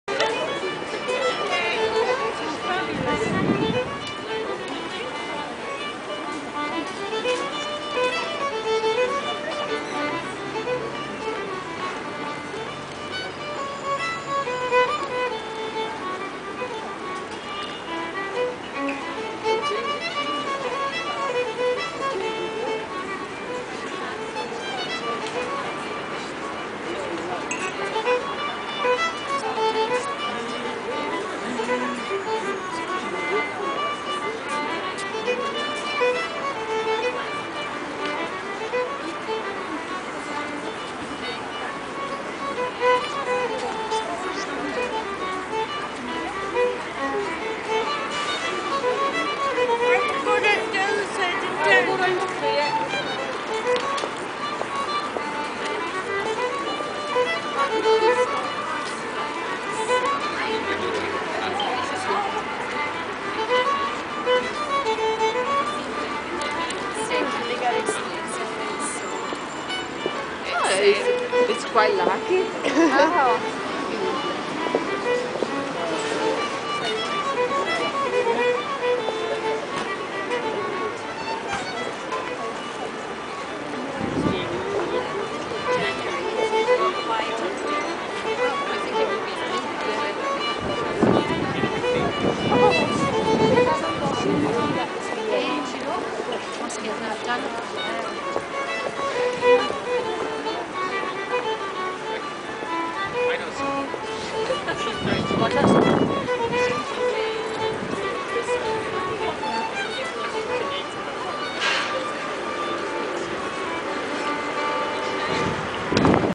Scottish lass dancing
Scottish Fiddler, Emma Chapman, next to the Scottish National Museum, Edinburgh inspires a young dancer